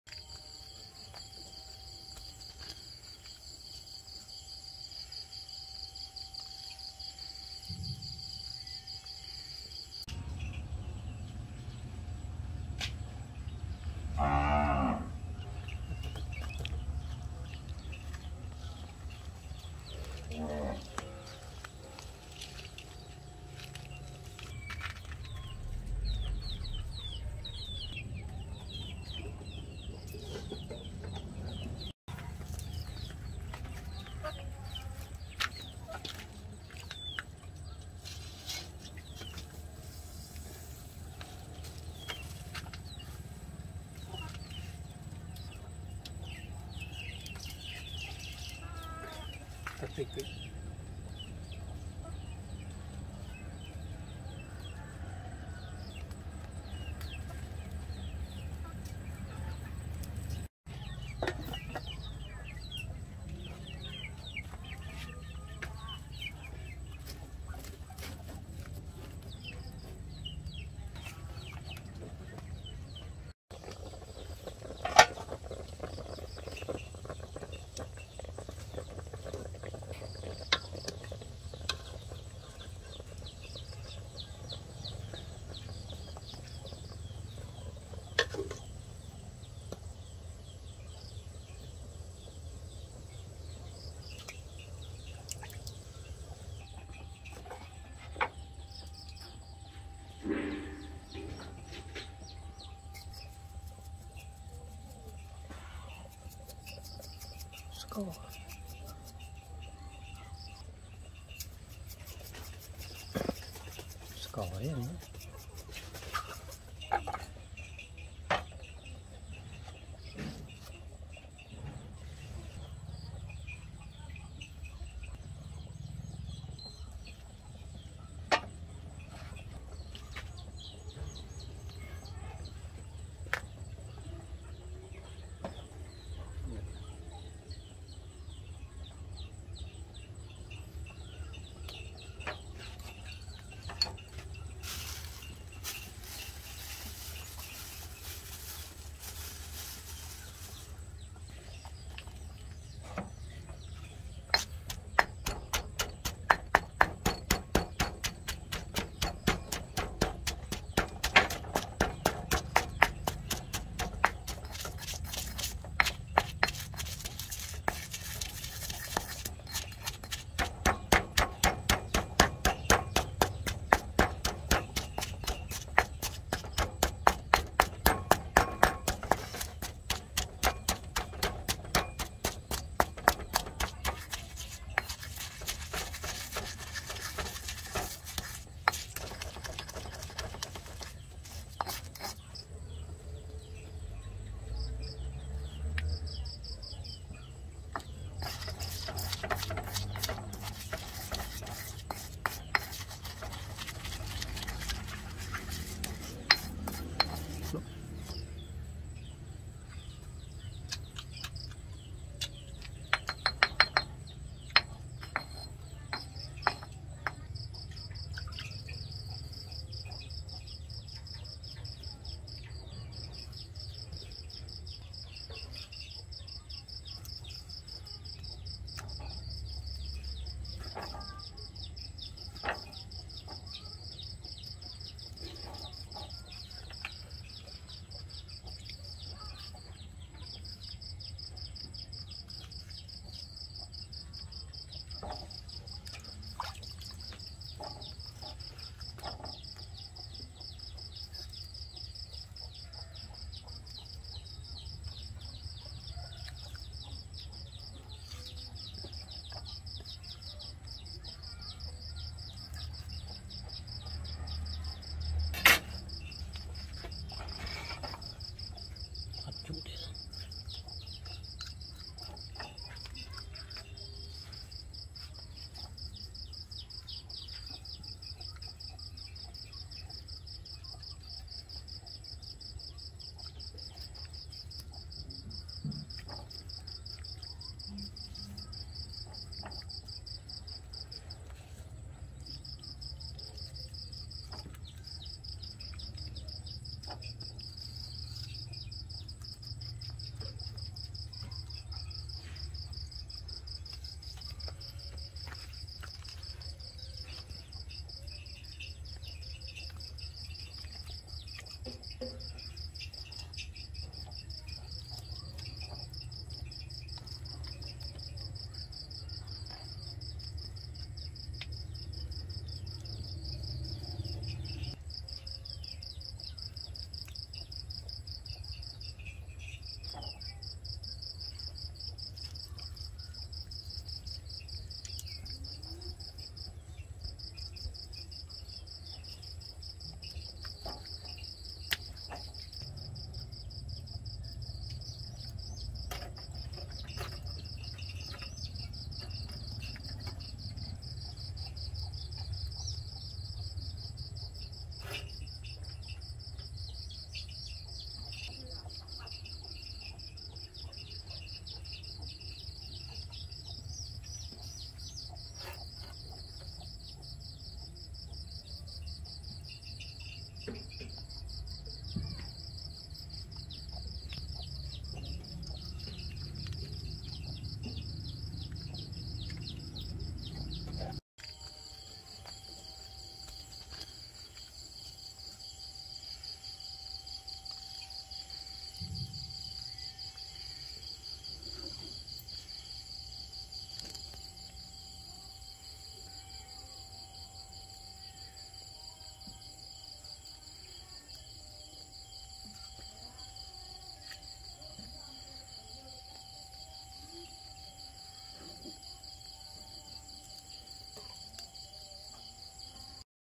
How to make grilled ears with salt and pepper to taste special
How to make grilled ears with salt and pepper to taste special​ Primitive jungle man